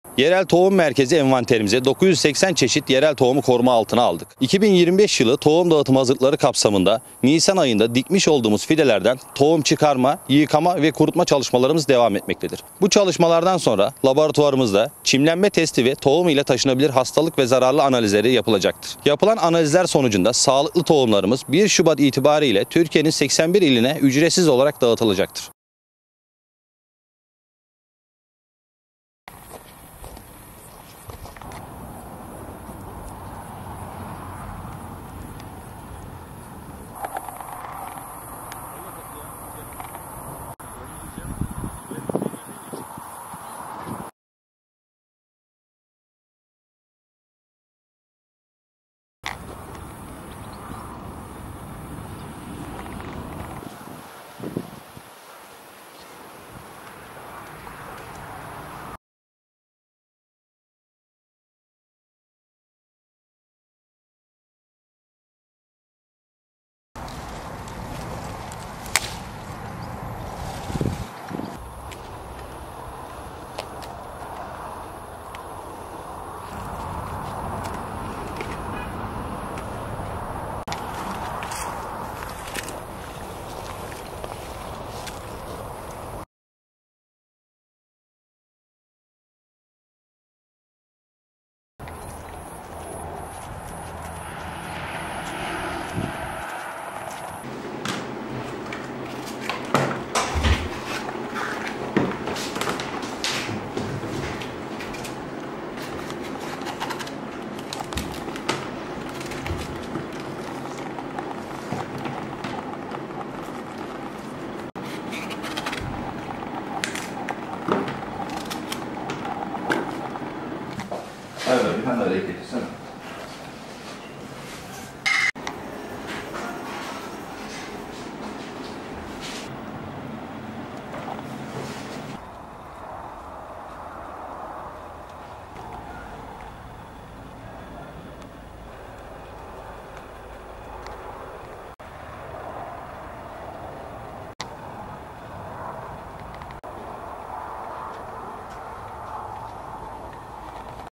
Muğla Büyükşehir 2025 Yerel Tohum Dağıtımı Hazırlıklarına Başladı
Türkiye’nin 81 iline bugüne kadar 19 milyon yerel tohum dağıtan Muğla Büyükşehir Belediyesi’nin Tıbbi-Aromatik Bitkiler ve Yerel Tohum Merkezi, 2025 yılı için ata tohum çalışmalarına başladı.